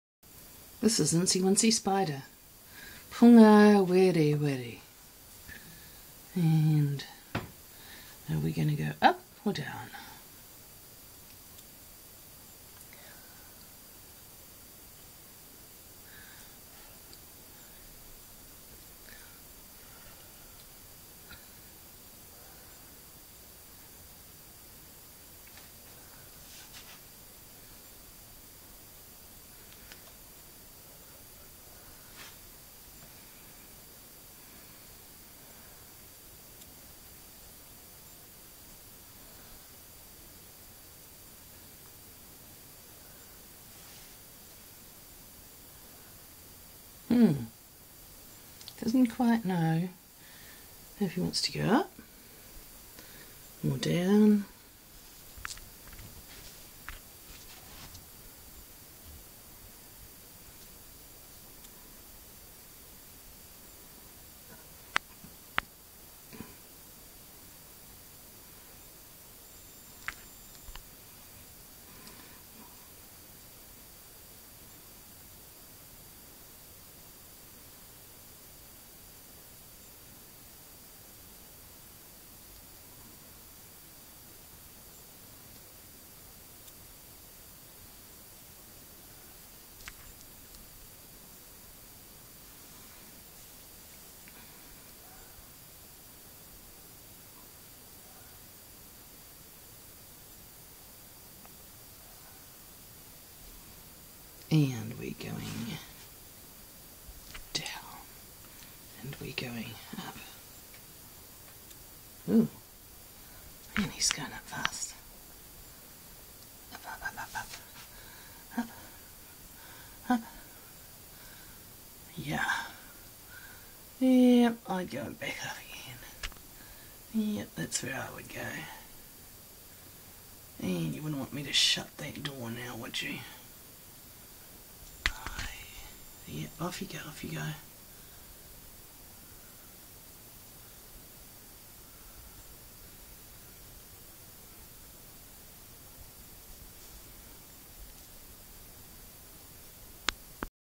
Spider - Pungawerewere
A spider who visited early one morning (12:40am!)